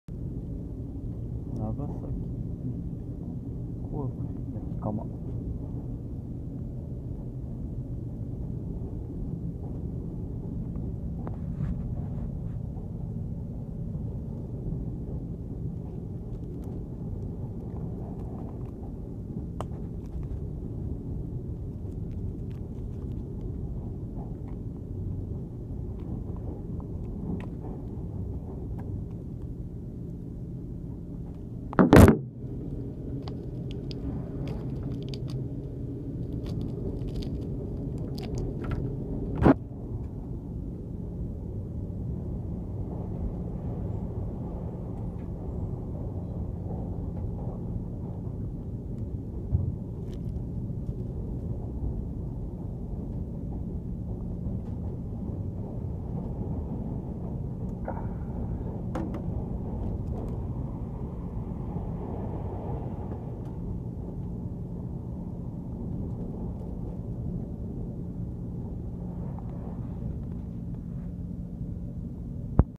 長崎 香福亭 焼かま 食べてみた(;ﾟДﾟ)!I tried to eat Nagasaki Kofuku-tei Yakama (; ° Д °)!
特急列車かもめに乗って長崎 香福亭 焼かまを食べてみた動画です。

Aboard the express train gull is a movie that tried to eat Nagasaki Kofuku-tei kilns.